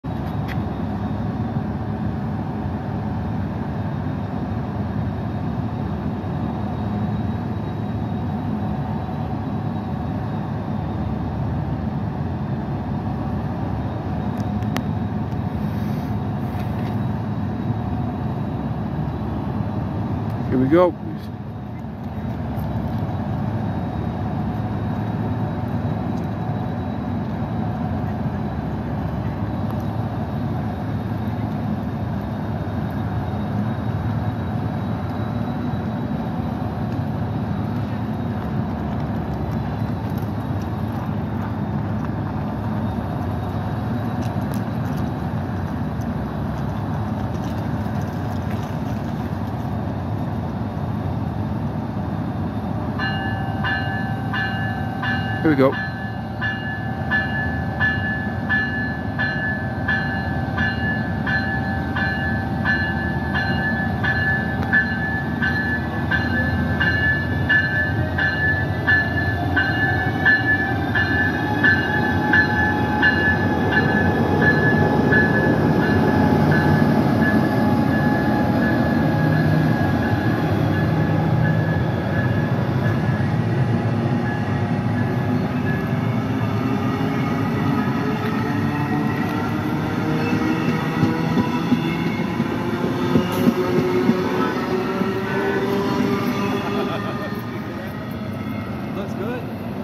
VIA Rail Siemens Charger Locomotive Departing Dorval Quebec 7/11/24
VIA Rail Siemens Charger Passenger Locomotive Departing Dorval Quebec For Montreal Quebec And Ponts in Eastern Quebec Canada 🇨🇦 And the VIA Rail Siemens Charger is a new State of the Art Passenger Rail Locomotive for VIA Rail